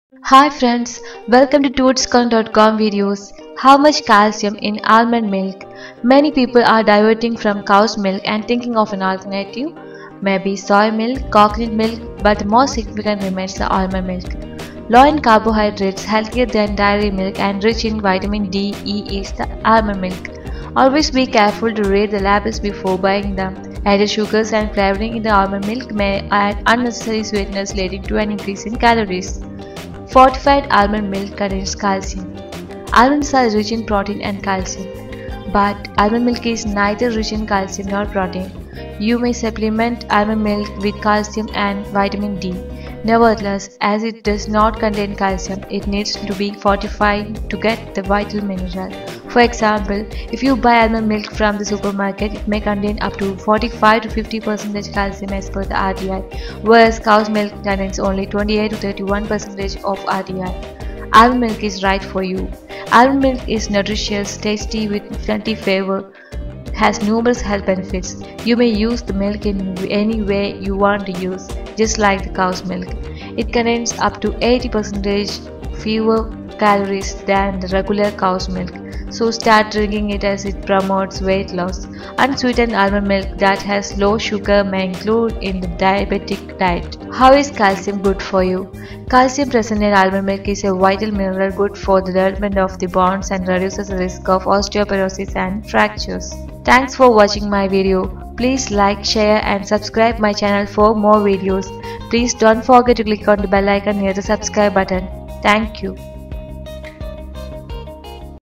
How much Calcium in Almond Milk? Is there Calcium in Almond Milk? Calcium Content in Almond Milk
Is almond milk fortified with calcium? Watch to know how much calcium is there in almond milk.